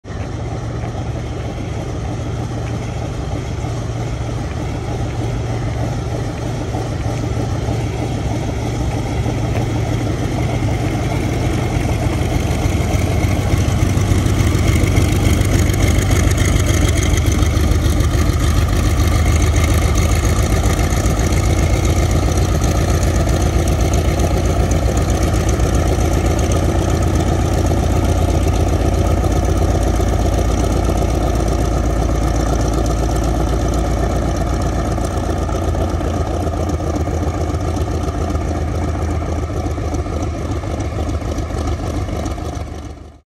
Vought F4U-7 'Corsair' taxis by this morning
Heading out of the Planes of Fame, what I thought was their F4U-1 is apparently The Erickson Aircraft Collection's F4U-7.